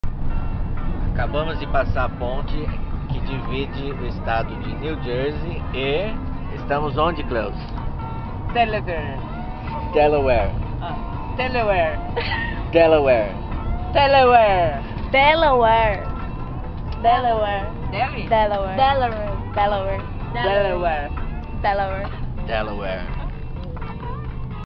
Turista Cleuza Delaware USA
A Turista Cleuza tenta pronunciar "Delaware" ao atravessar uma ponte e adentrar aquele estado nos EUA.